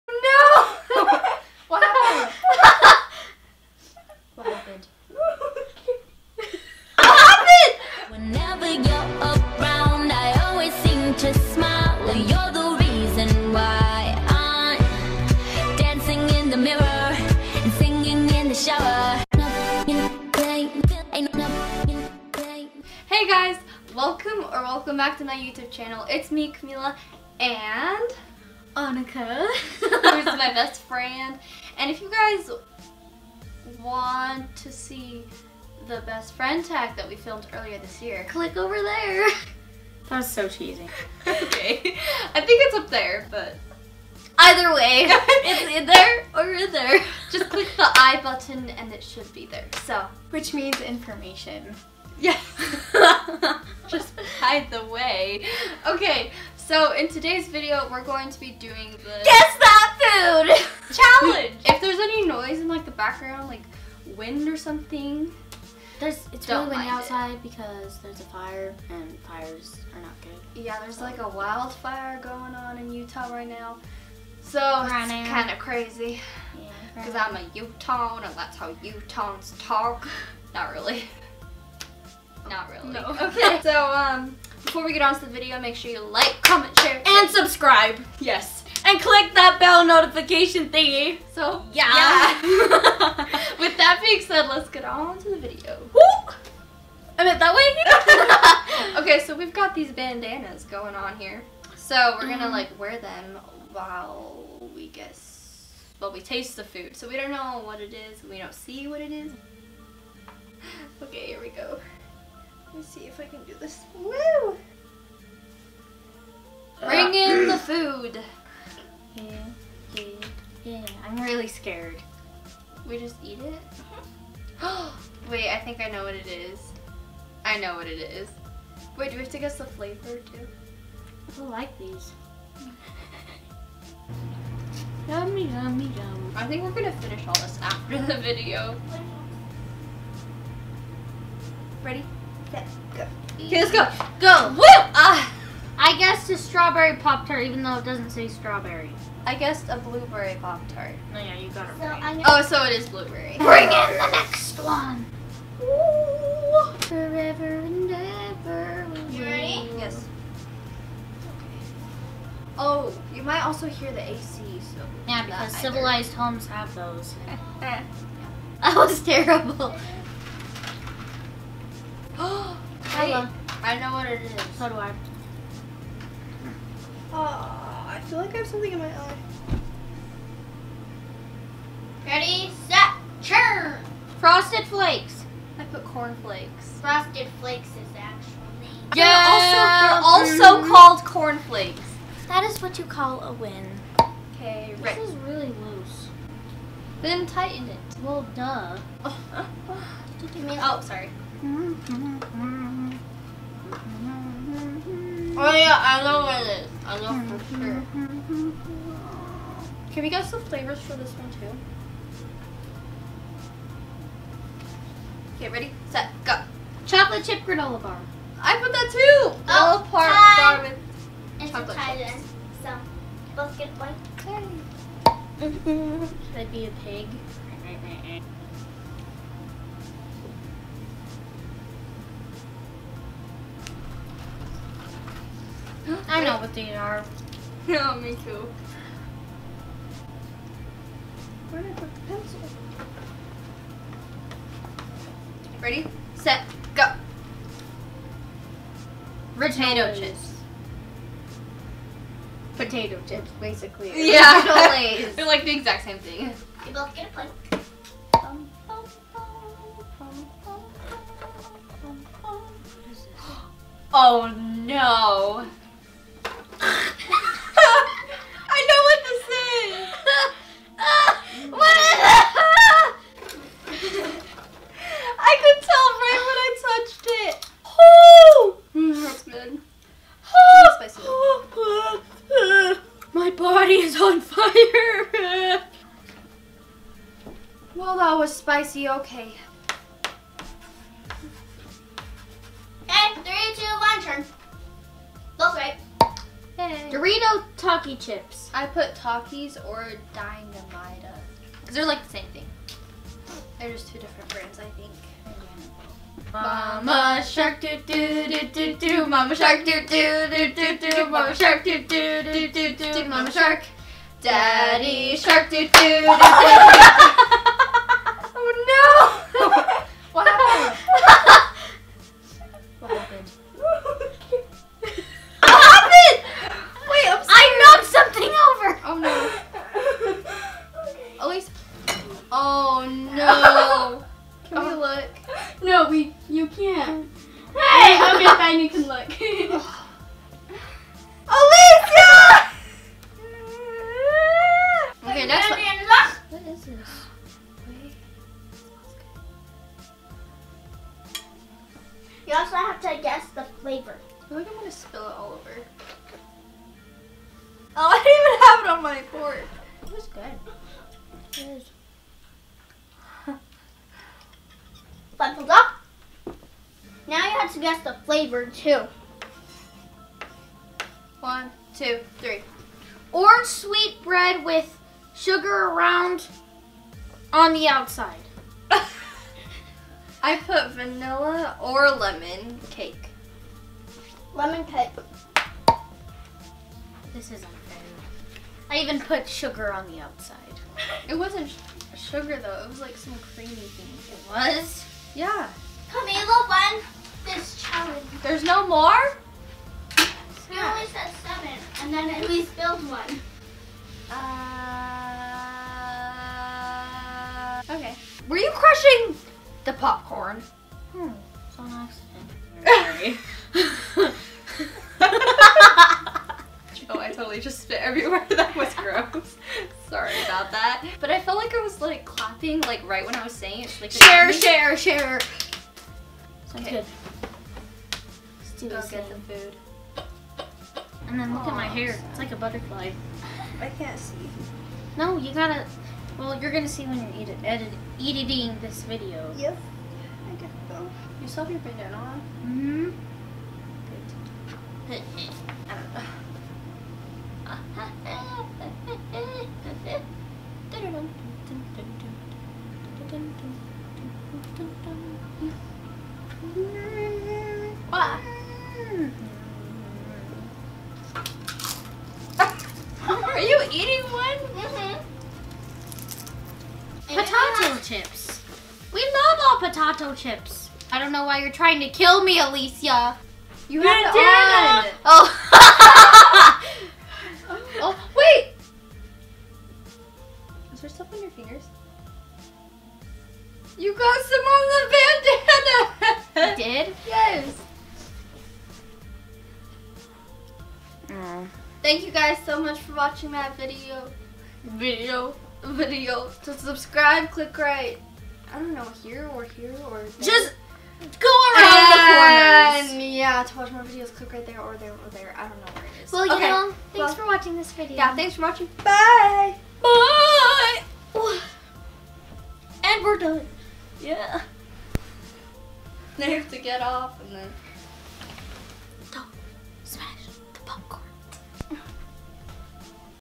GUESS THAT FOOD CHALLENGE!| ItsMilaCamila
Hey! In this video my friend, Aunika, and I do the "guess that food challenge!" We are blindfolded while we guess what certain foods are. Watch the video to see our hilarious bloopers and who won!

-Camila :)